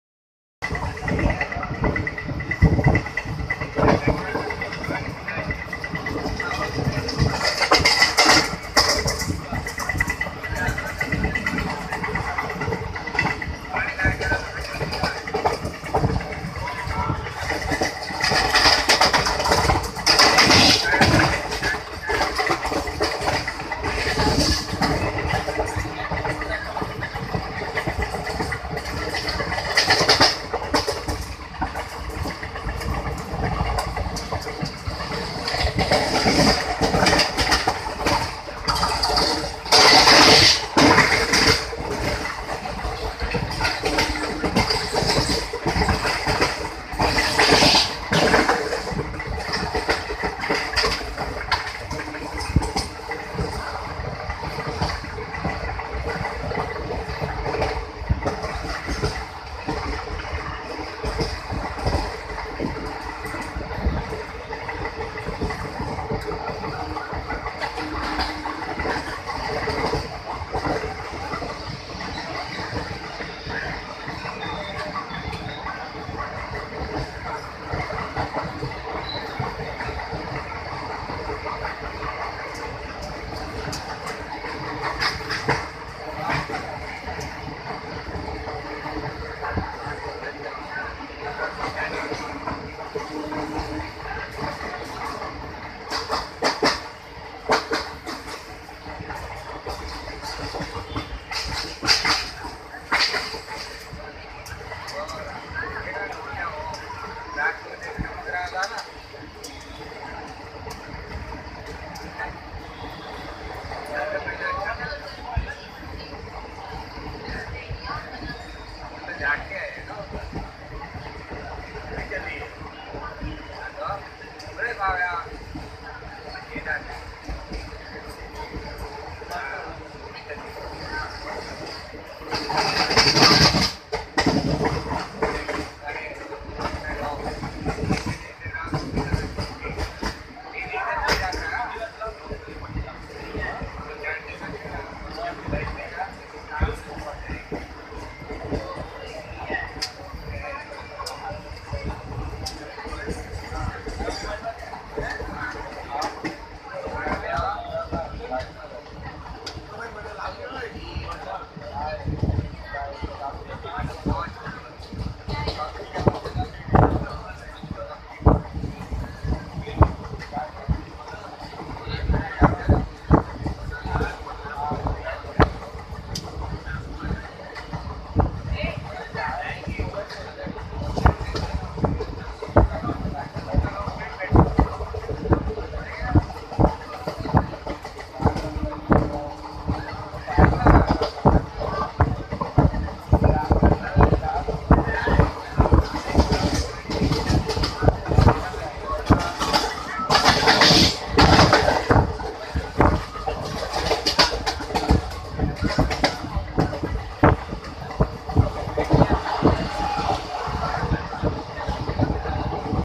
Shri Jivdaani Mata... Gadh Darshan... while travelling by train...
Train passing through Virar rly station.